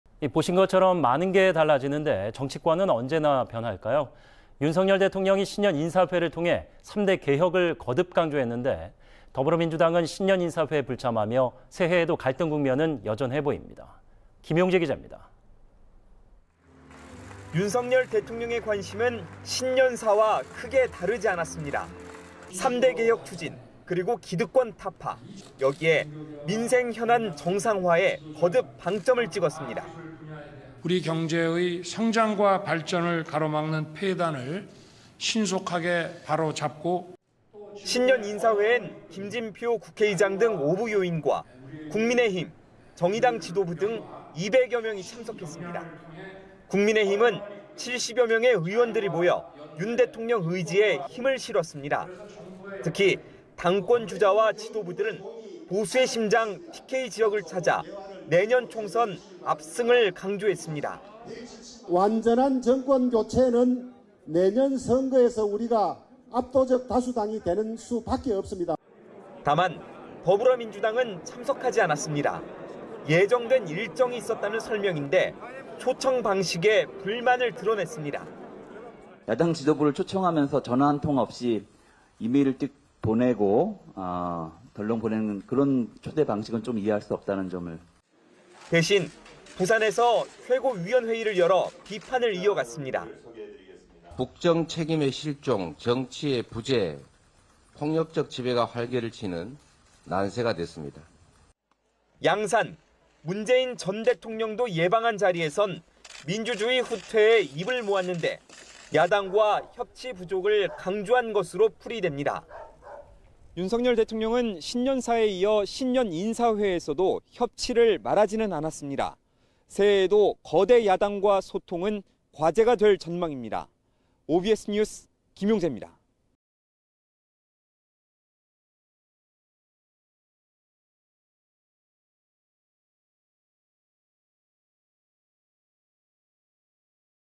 3대 개혁 거듭 강조한 윤 대통령... 민주당은 신년사 불참
보신 것처럼
많은 게 달라지는데,
정치권은 언제나
변할까요?
윤석열 대통령이
신년 인사회를 통해
3대 개혁을
거듭 강조했는데,
더불어민주당은
신년인사회에 불참하며
새해에도 갈등 국면은
여전해 보입니다.
김용재 기자입니다.